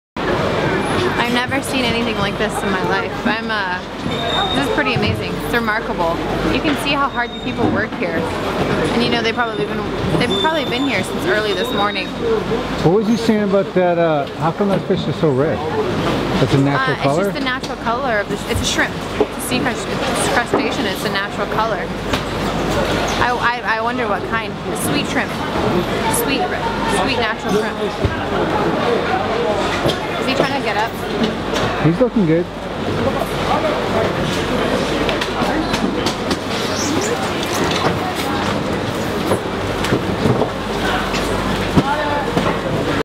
The Tsukiji Fish Market
I was so impressed with the hard working individuals at The Tsukiji Fish Market, this is just truly a once in a lifetime experience to be with my son and seeing a whole different culture and work ethic with him. Not to mention seeing the world of exotic fish and cuisine.